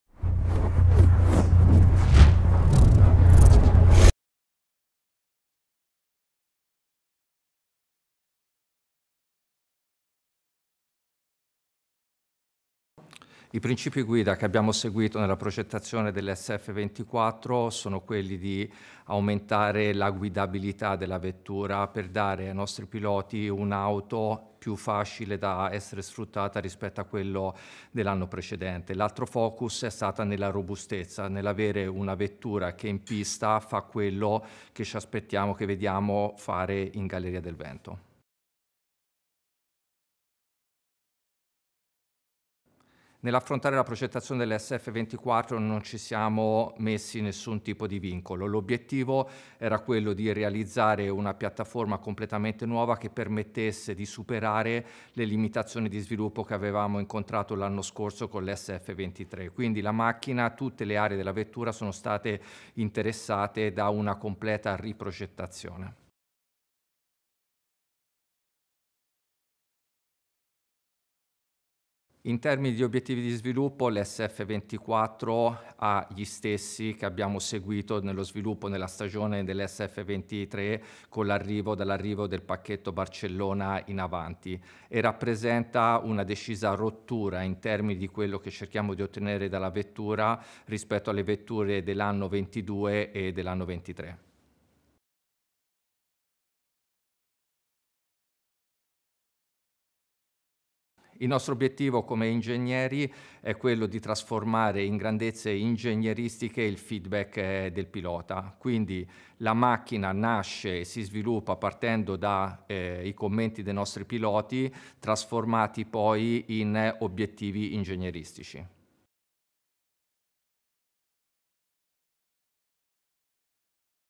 F1 | Ferrari SF-24, parla Enrico Cardile
Il Q&A di Enrico Cardile, Direttore Tecnico Chassis, per la presentazione della Ferrari SF-24, la monoposto per il mondiale di F1 2024